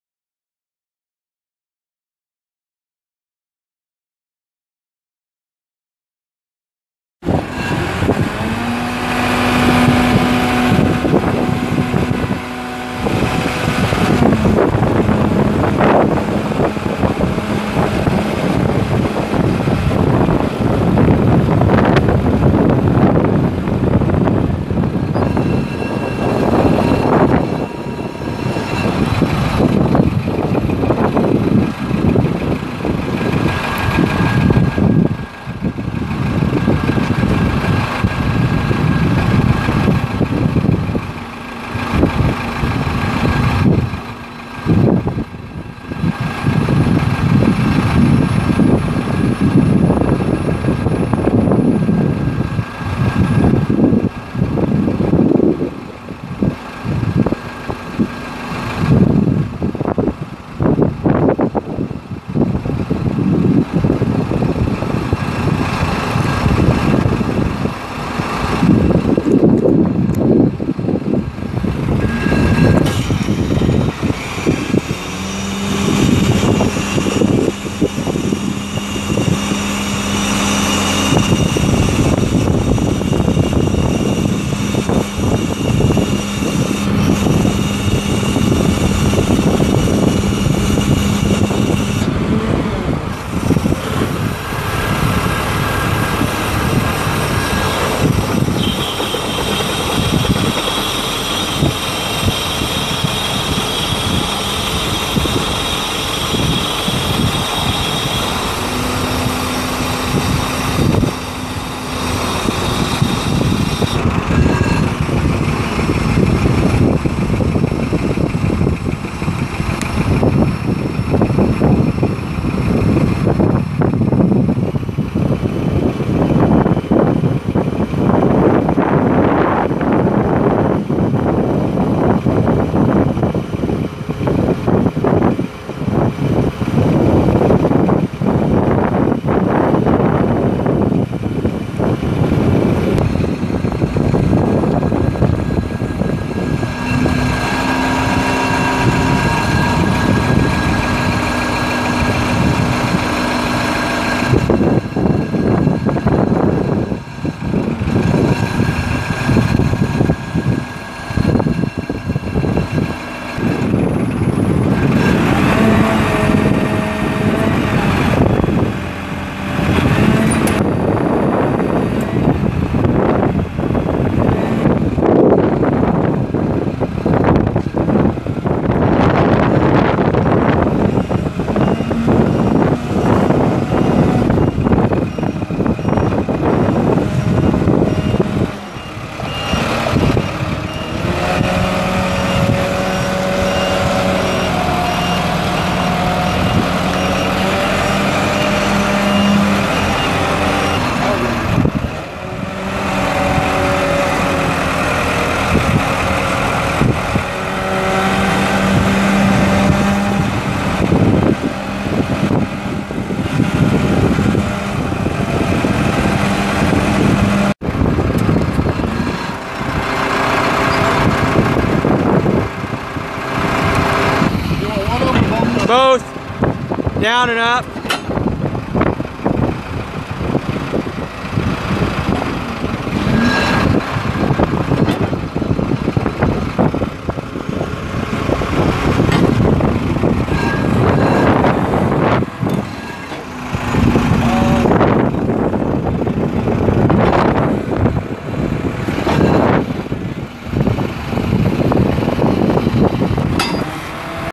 1999 Sky Trak 10054 telehandler for sale | no-reserve Internet auction March 29, 2017
1999 Sky Trak 10054 telehandler
2,945 hours on meter
Cummins 4BT 3.9L four cylinder turbo diesel engine
Serial 45833758
Hydrostatic transmission
3F-3R gears
OROPS
Four wheel steering
10,000 lbs max lift capacity
56' max lift height
40' max reach length
Four stage boom
53" fork length
Tilt carriage
Hydraulic outrigger
17.5x25 tires
Serial 10075
Unit# 52008